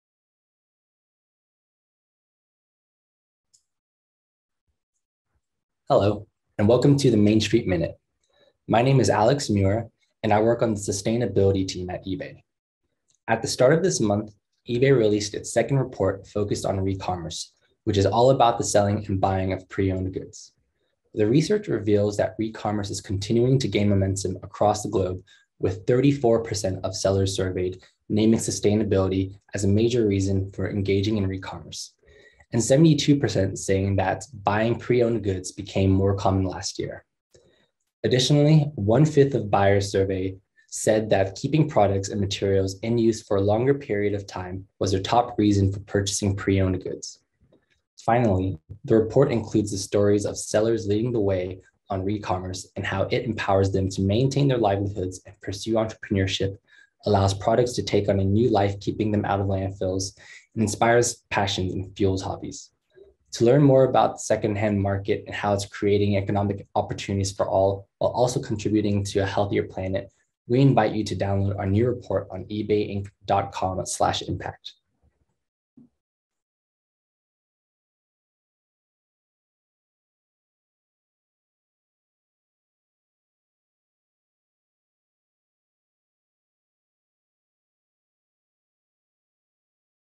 Main Street Minute - Episode 15: eBay Launches 2021 Recommerce Report
This episode of the Main Street Minute highlights eBay's new report on Recommerce - or the selling and buying of pre-owned goods – and how the secondhand market is creating economic opportunities for all, while also contributing to a healthier planet.